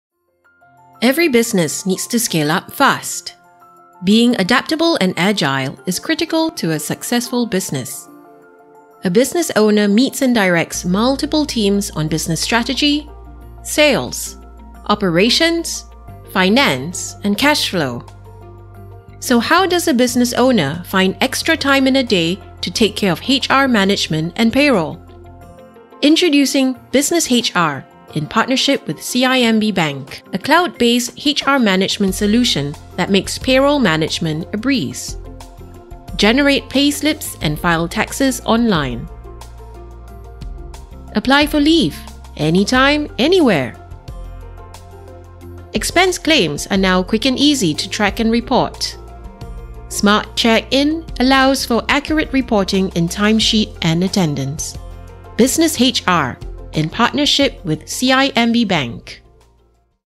CIMB x Business HR
At CIMB, we continuously strive to provide solutions and work with partners to meet your evolving business needs. BusinessHR, our partner solution offers a cloud-based HR management solution that digitizes your HR processes to provide your business with convenience & cost savings. Enjoy complimentary usage up to 3 months* as a CIMB customer today!

CIMB Bank Berhad (13491-P)